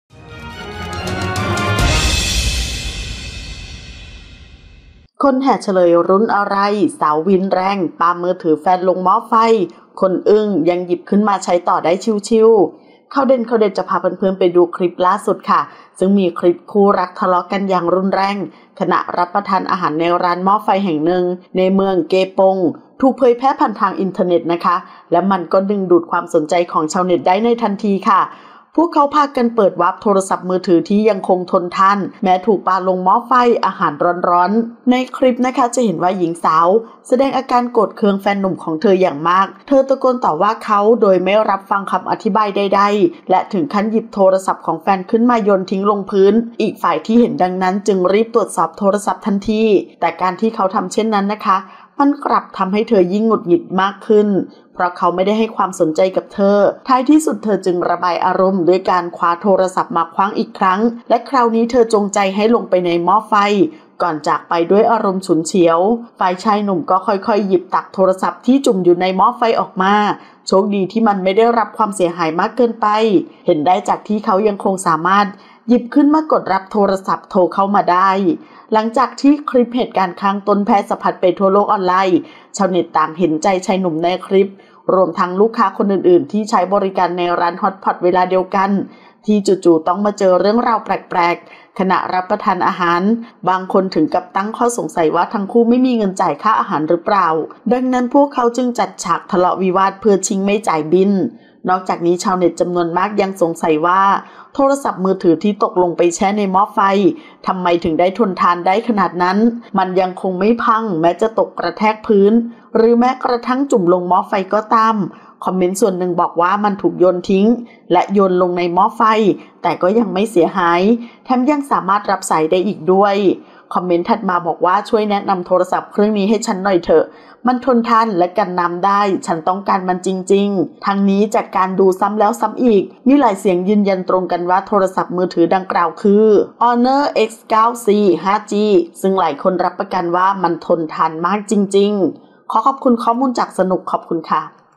คนแห่เฉลยรุ่นอะไร? สาววีนแรง ปามือถือแฟนลง "หม้อไฟ" คนอึ้งยังหยิบขึ้นมาใช้ต่อได้ชิลๆ : RECAP
00:00 - Intro
00:10 - News
01:00 - Minute